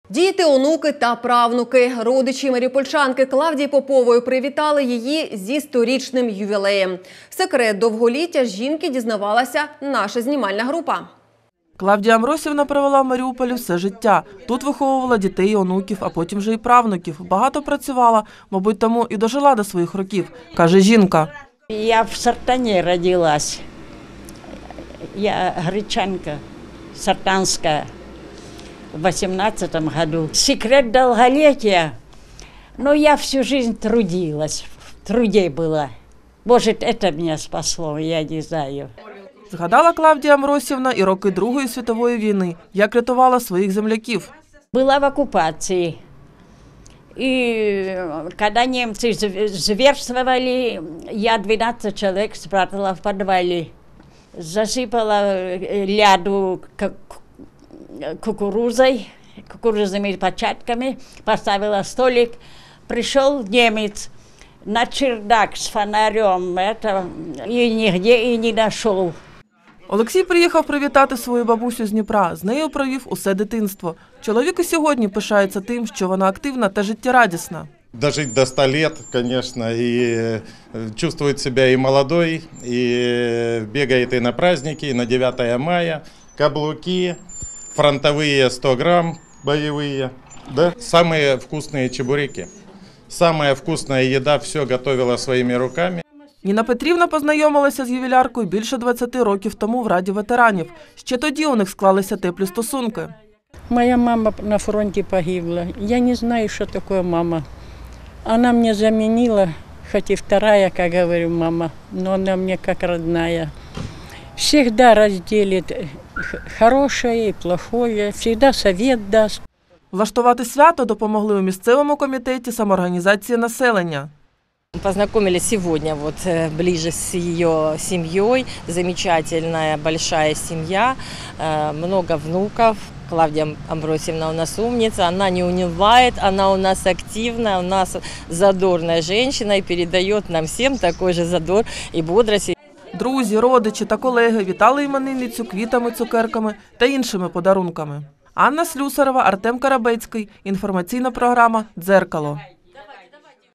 25.09 | Секрет довголіття від 100-річної маріупольчанки
Діти, онуки та правнуки. Родичі маріупольчанки Клавдії По-пової привітали її зі сторічним ювілеєм. Секрет довголіття жінки дізнавалася наша знімальна група.